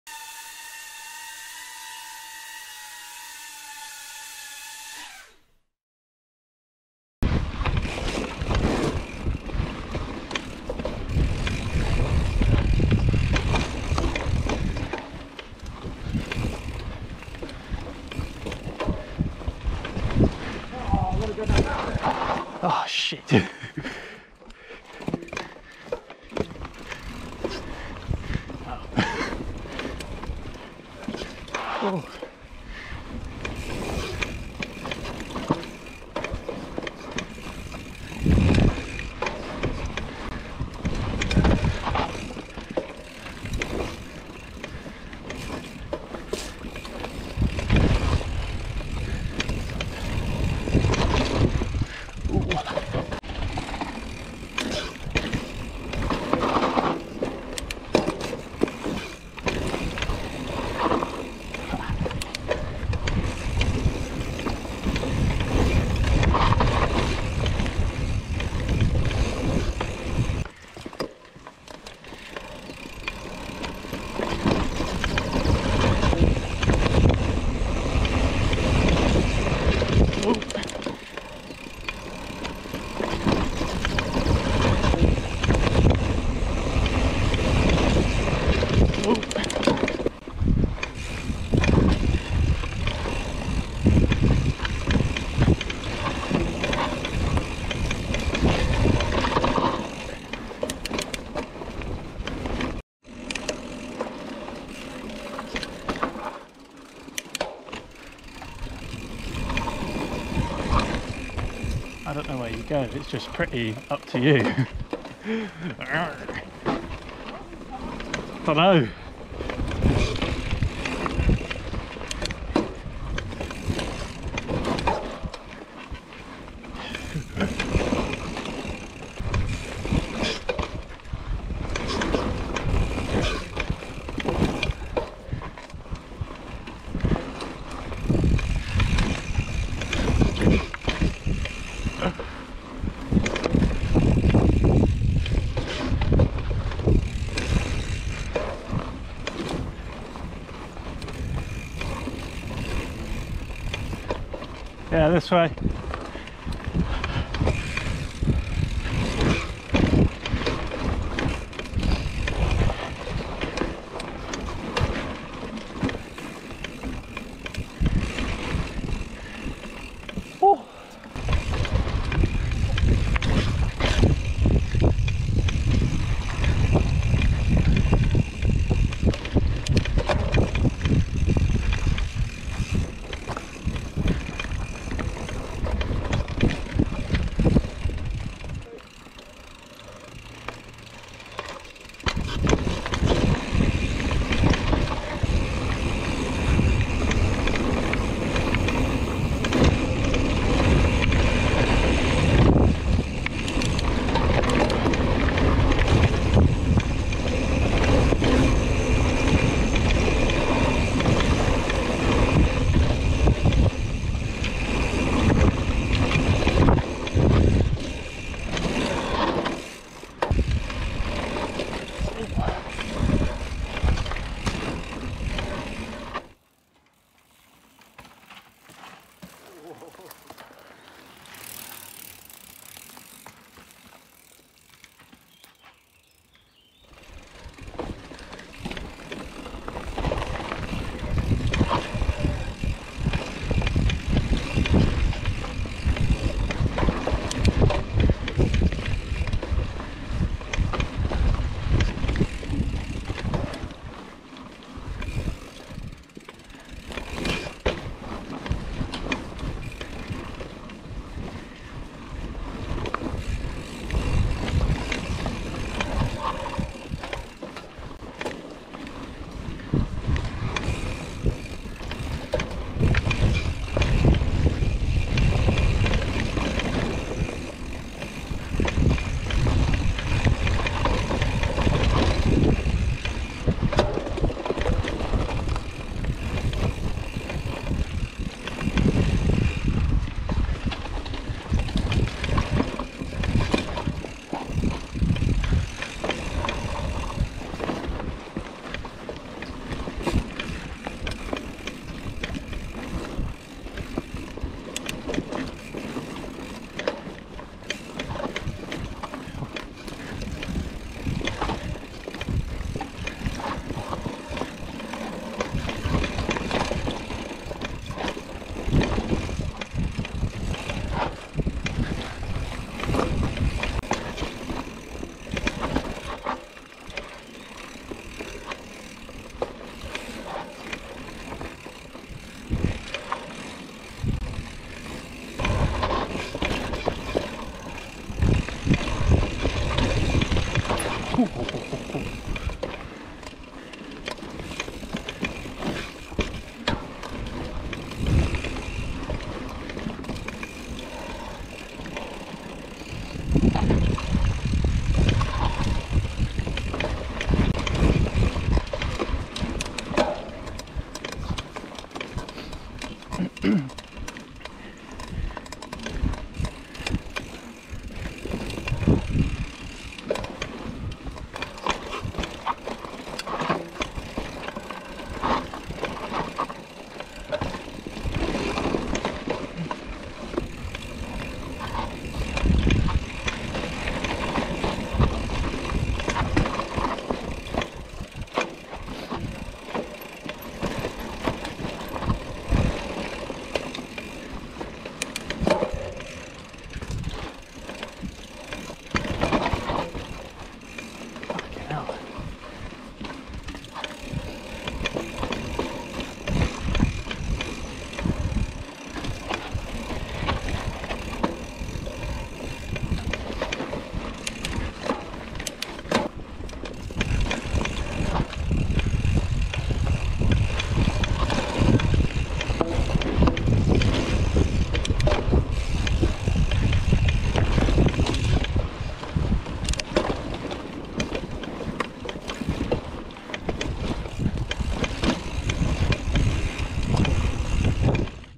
Woburn Sands Londslade Trail GoPro POV
A POV ride around the Longslade Trail at Woburn Sands in Milton Keynes.

Both of us riding hardtails.